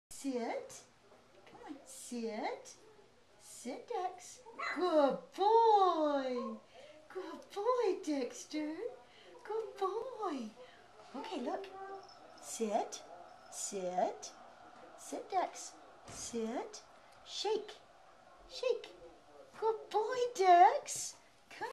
Dexter the Miniature Schnauzer Puppy Learning to Sit
Dexter is learning to sit here. We are also starting to do shake with him. It is so cute! Check out our blog for more pictures and videos of adorable puppies!